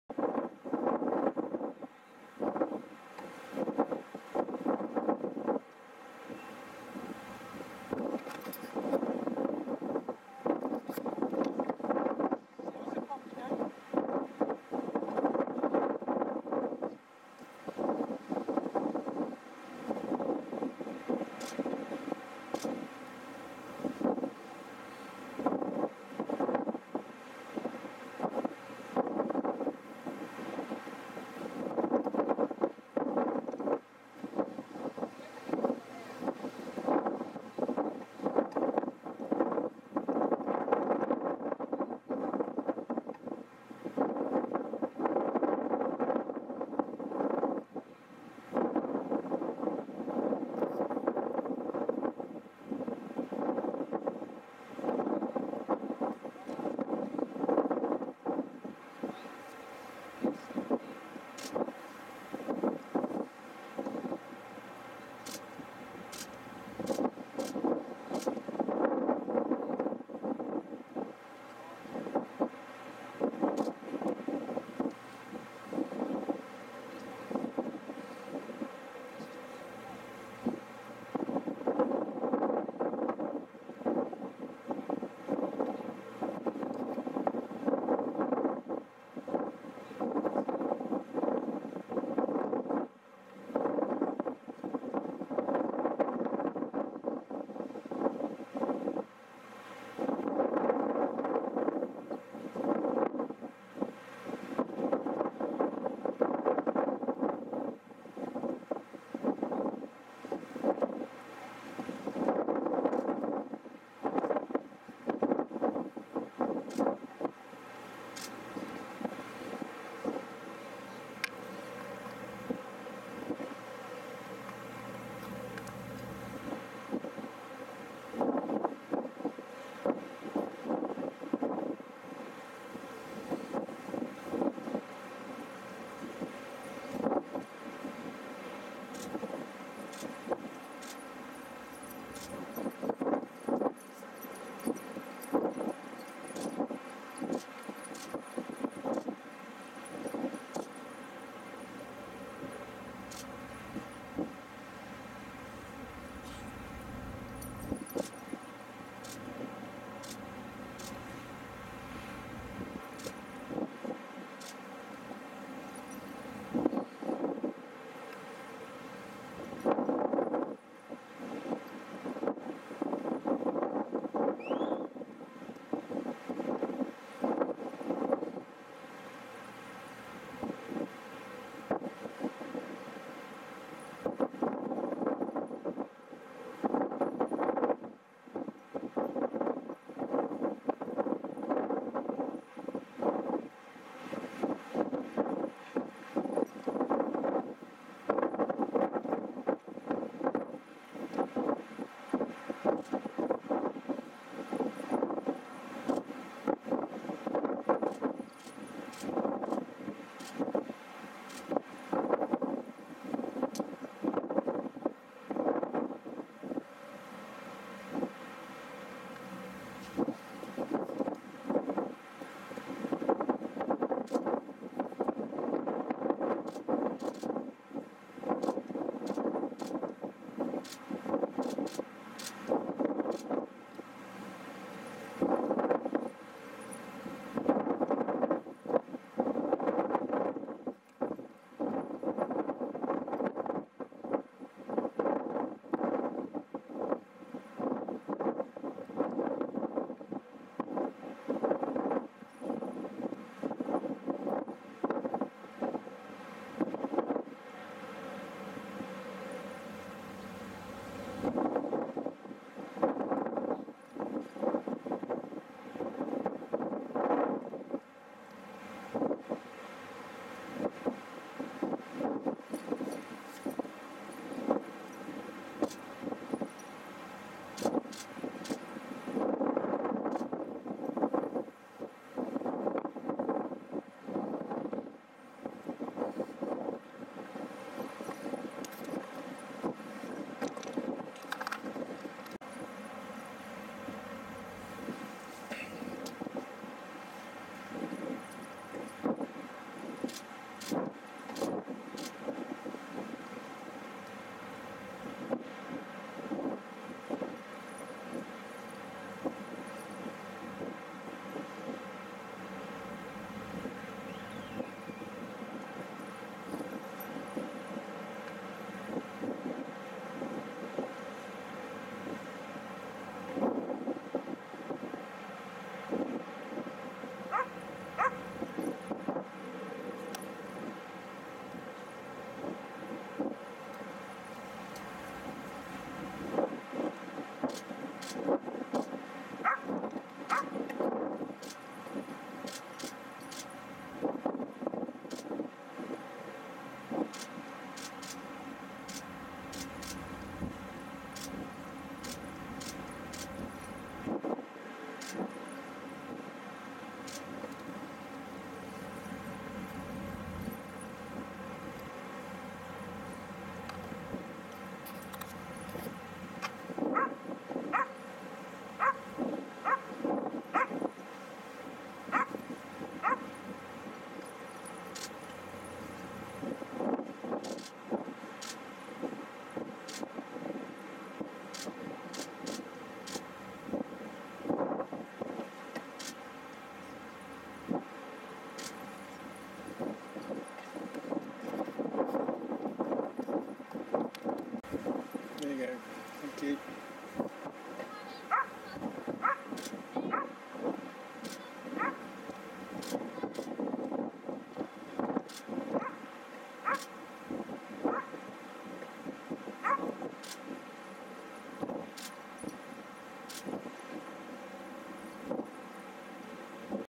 USS Sioux City LCS-11 Departing Plymouth UK 7 Sept 2022 pt3
USS Sioux City LCS-11. Crossing Plymouth Sound and the Breakwater.  Passing first RFA Mounts Bay L3008 with a Merlin helicopter on her deck and then HMS Albion L14. Apologies for the dogs, don't own one and never will. Panasonic HC-VXF1 4k camcorder.
00:00 Appearing from farside (east) of Drakes Island.